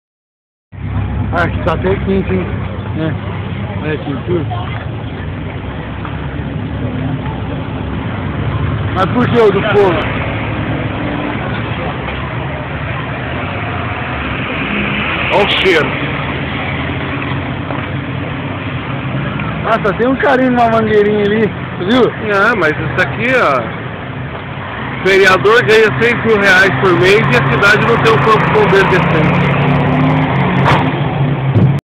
Incêndio em Ourinhos
Nosso corpo de bombeiros está equipado para combater fogo de grandes proporções???

Essa não é uma questão que nossas autoridades deveriam se preocupar?